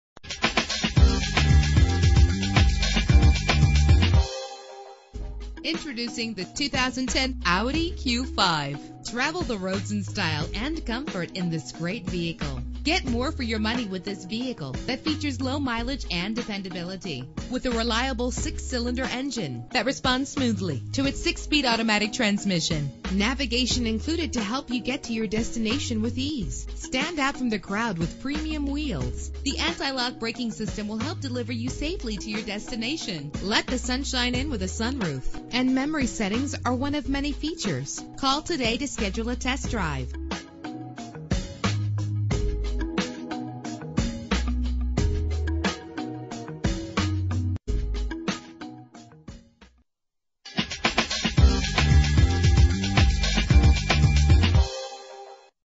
2010 Audi Q5 - Norwalk CA
Year: 2010
Make: Audi
Model: Q5
Trim: 3.2 quattro Premium
Engine: 3.2 liter 6 cylinder 24 valve
Transmission: 6-Speed Automatic
Color: Brilliant Black
Mileage: 9690
Address:
10915 Firestone Blvd